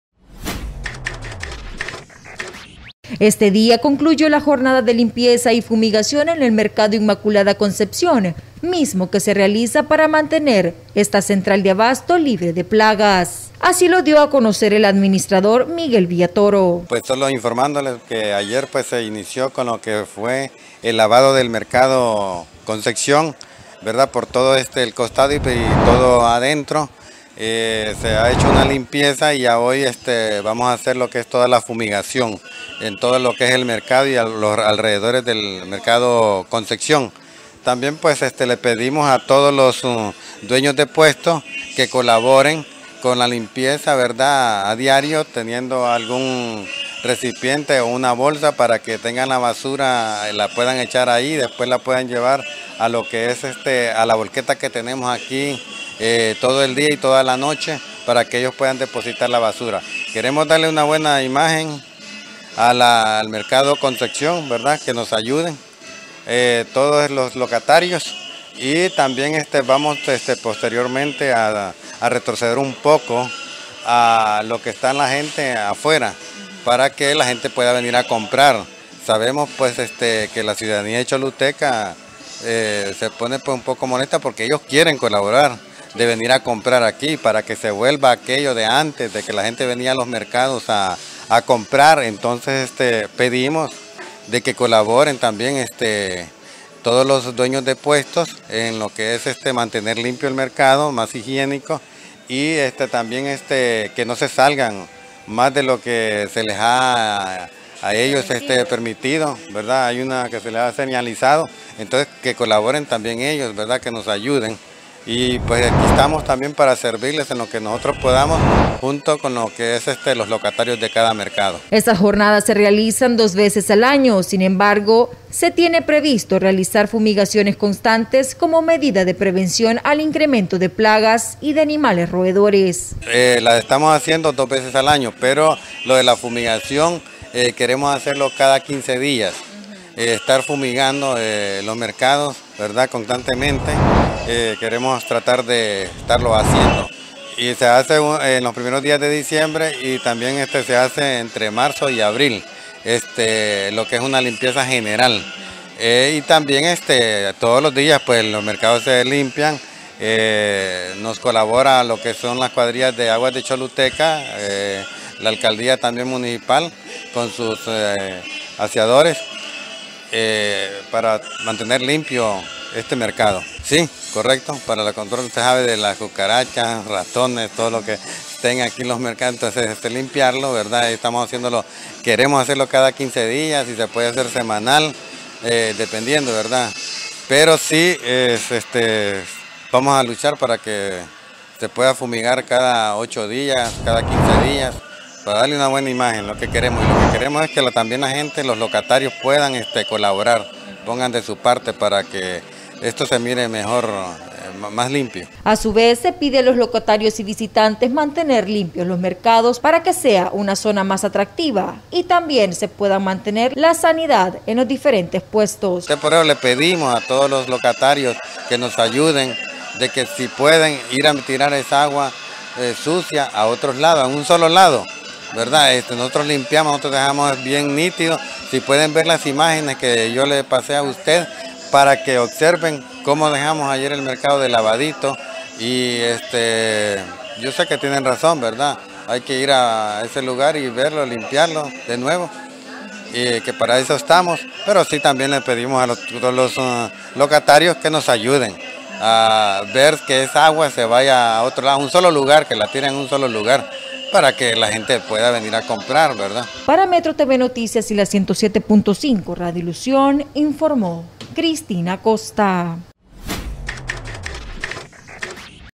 CONCLUYE JORNADA DE LIMPIEZA Y FUMIGACION EN EL MERCADO INMACULADA CONCEPCION
Lametrohn.com